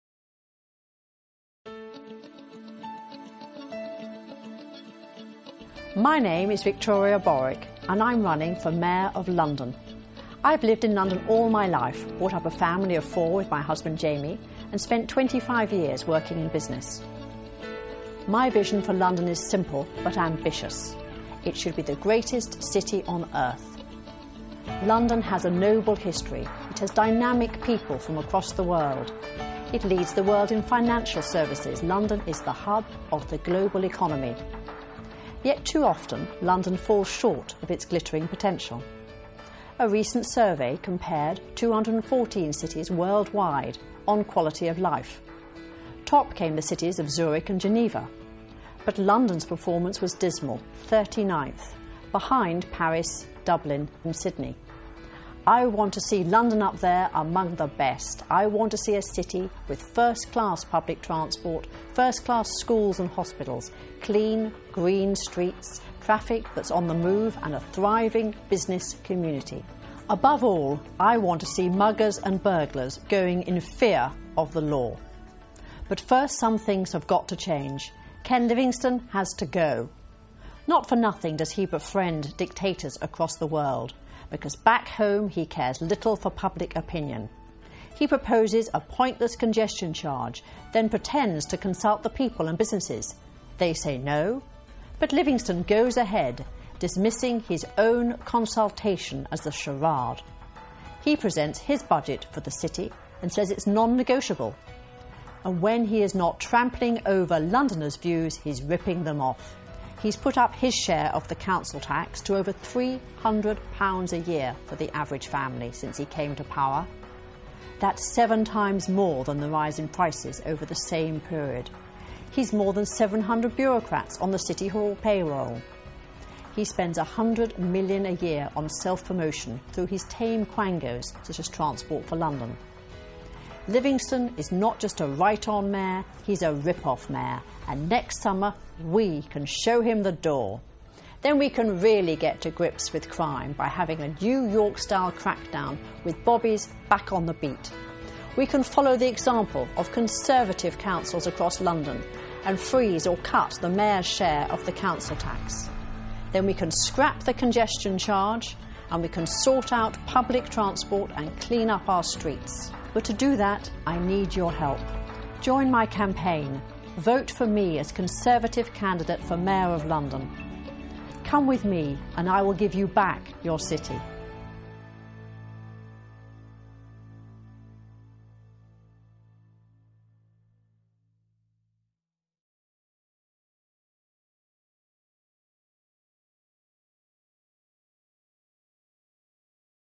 Victoria Borwick
First campaign video from Victoria Borwick, a candidate for the Conservative nomination for the London Mayorlty